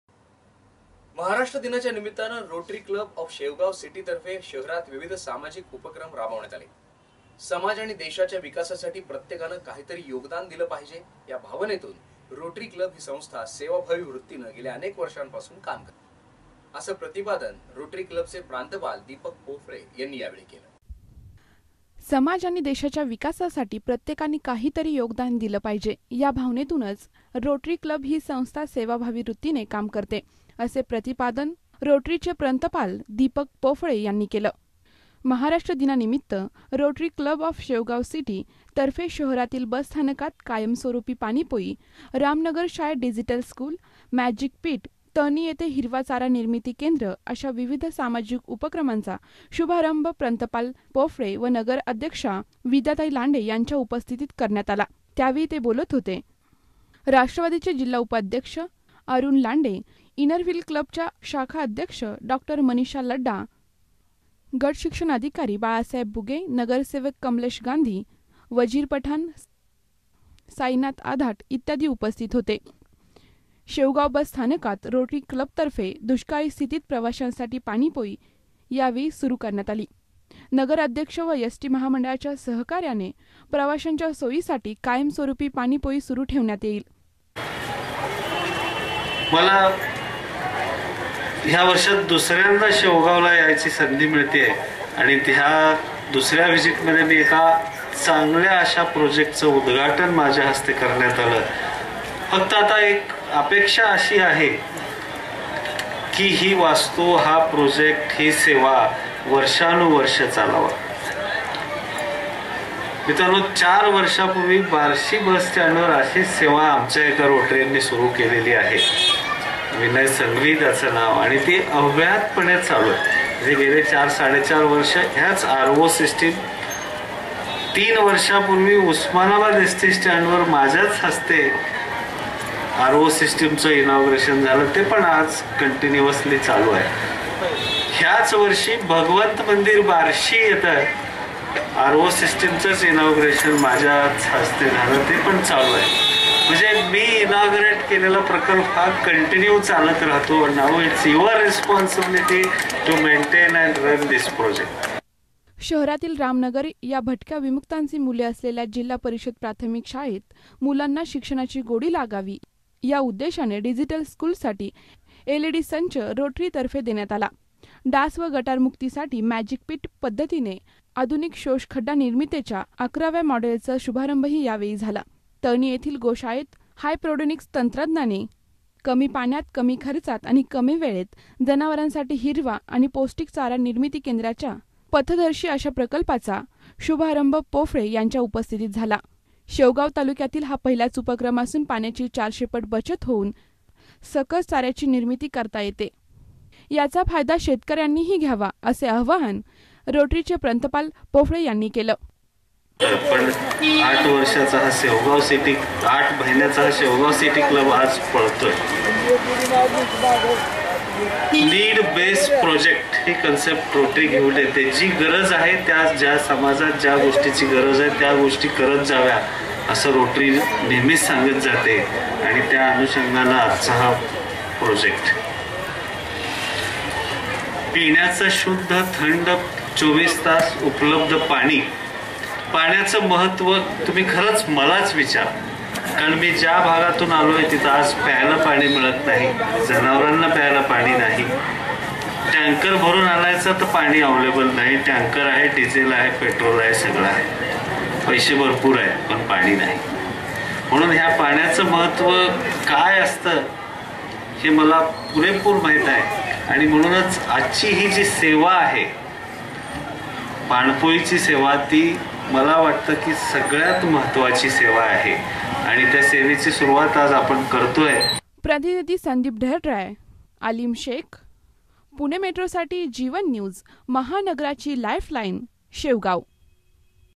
महाराष्ट्रदिना निमित्त रोटरी क्लब ऑफ शेवगाव सिटी तर्फे शहरात विविध सामाजिक उपक्रम राबवले गेले
महाराष्ट्रदिना निमित्त रोटरी क्लब ऑफ शेवगाव सिटी तर्फे शहरात विविध सामाजिक उपक्रम राबवले गेले. समाज व देशाच्या विकासासाठी प्रत्येकाने काही तरी योगदान दिले पाहिजेयाच भावनेतून रोटरी क्लब ही संस्था सेवाभावी वृत्तीने काम करते असे प्रतिपादन रोटरी क्लबचे प्रांतपाल दीपक पोफळे यांनी केले.